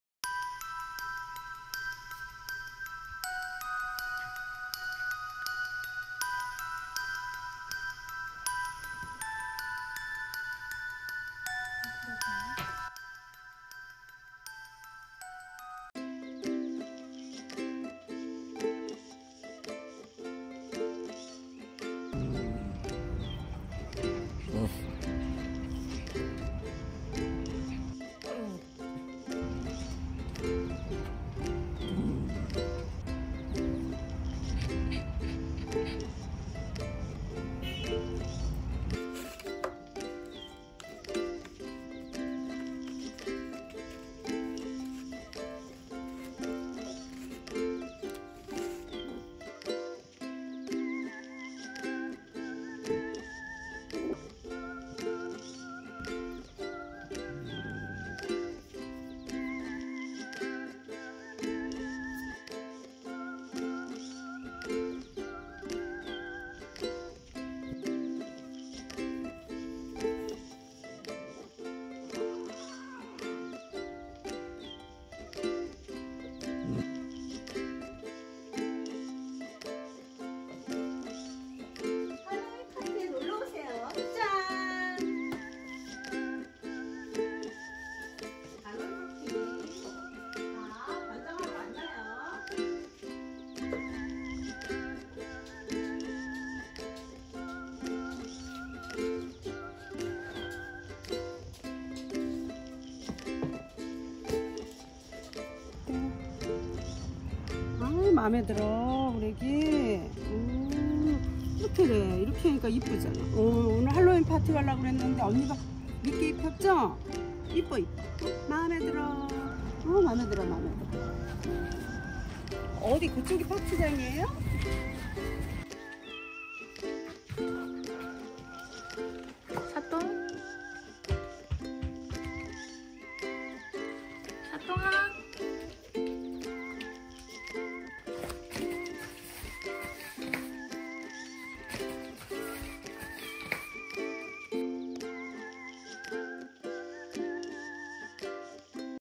🎃Trick or Treat! 할로윈 홈파티를 준비하는 검은고양이 샤똥이👻
할로윈 홈파티를 준비하는 검은고양이 샤똥이💕
턱시도, 검은고양이가 할로윈에 잘 어울리죠?
Happy Halloween!
안전거리 유지하고 파티해용! ㅎㅎㅎ

샤똥이가 할로윈을 맞이해서 홈파티를 하려고
작은 솜방망이 손으로 하나씩 만들어요 ㅋㅋㅋㅋ
코스튬, 음식, 장식품 등등
샤똥이가 파티를 준비하는 영상입니다.

사랑하는 빠삐로이샤똥💕
항상 행복하고 건강하게 오래오래 함께하자

영상을 퍼가실때는 유튜브 링크를 통해 공유해 주시고
유튜브 채널 출처를 꼭 남겨주세요!!